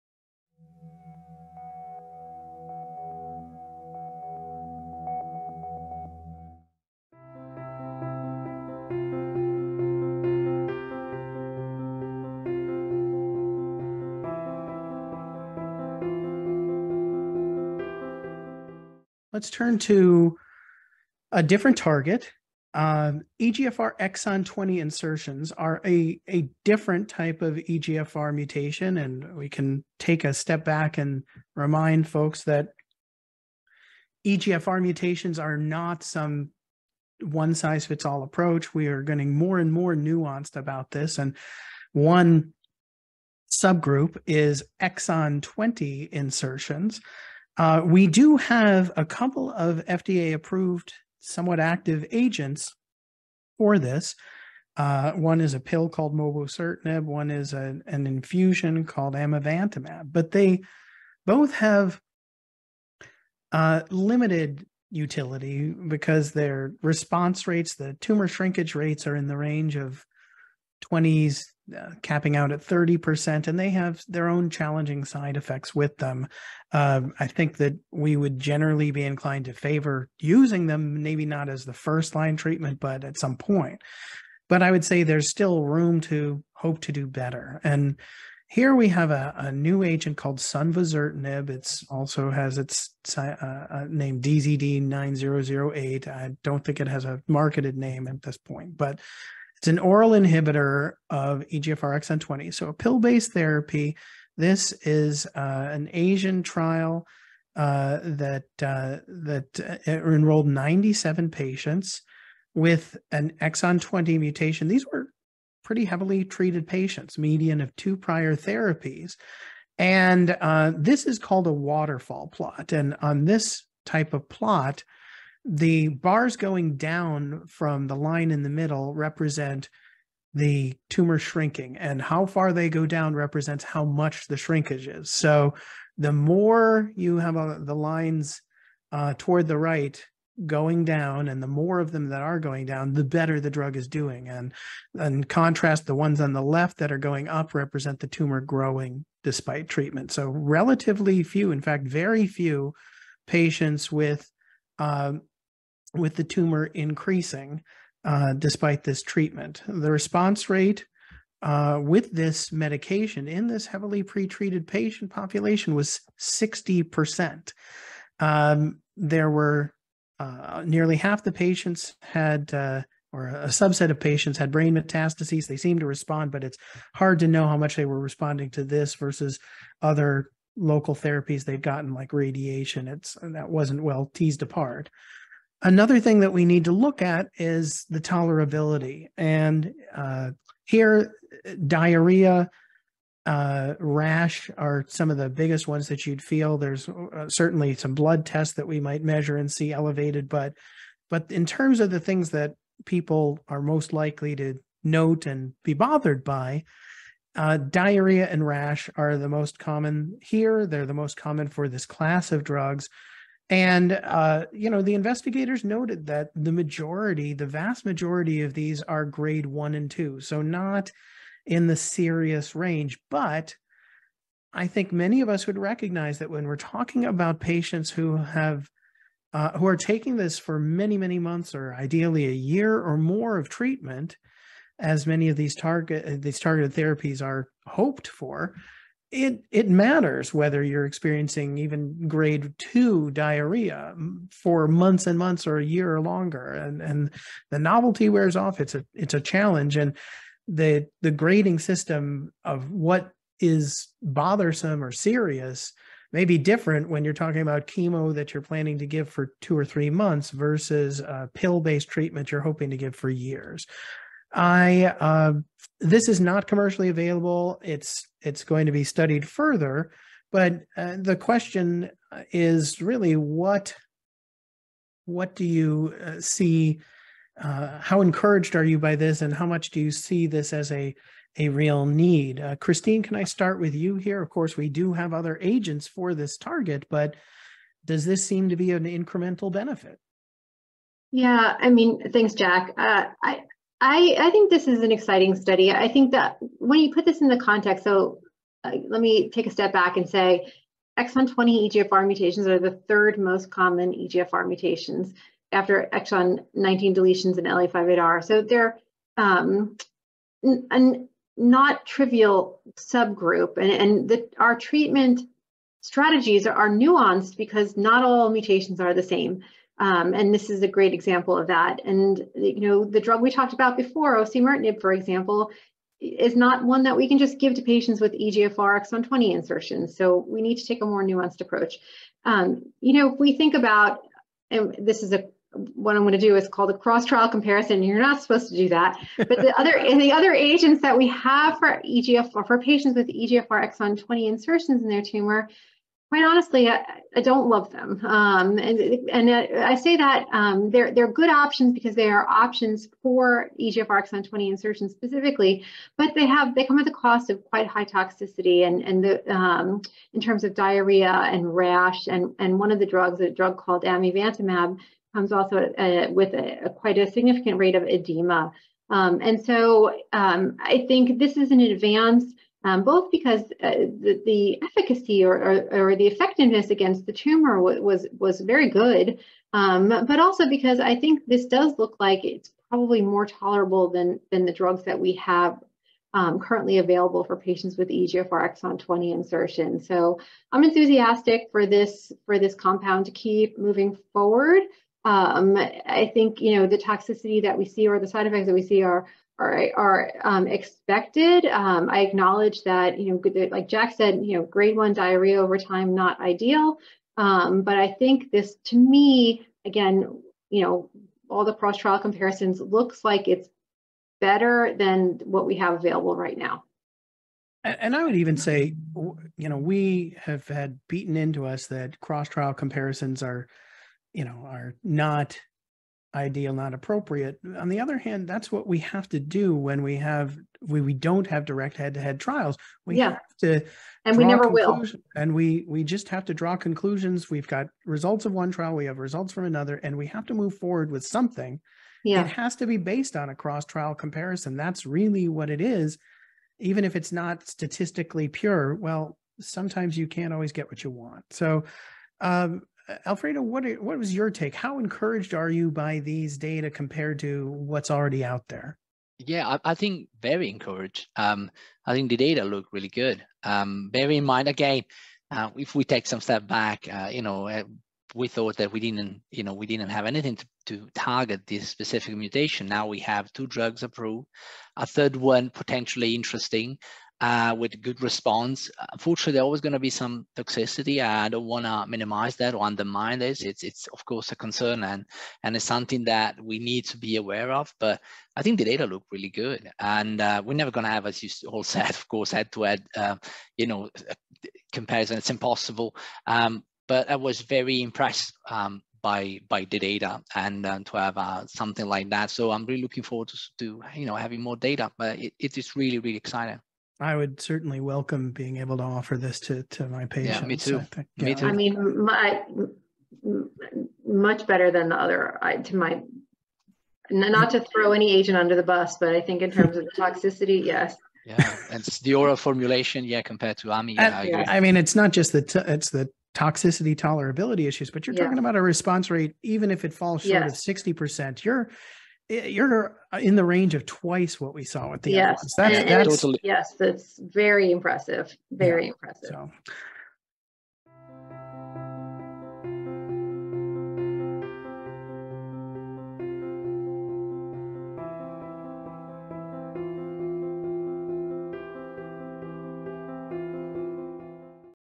Sunvozertinib & EGFR Exon 20 Insertion NSCLC-Leading Developments & Current Questions in Lung Cancer
For this year's post-ASCO Lung Cancer panel discussion, Drs. H.Jack West, Oncologist with City of Hope Comprehensive Cancer Center, and Founder and President of GRACE, Christine Lovly, Associate Professor of Medicine, Division of Hematology-Oncology, and Dr. Alfredo Addeo, Medical Oncologist, department of Oncology at the University Hospital of Geneva (HUG), discuss the most current and emerging treatment information recently presented at ASCO.

Drs. West, Lovly, and Addeo review key information from 2023 ASCO on lung cancer developments, including the results of several trials.

In this video, Drs. West, Lovly, and Addeo look at data on Sunvozertinib's use against EGFR Exon 20 NSCLC.

License #: 2LN6RROJA08RATWY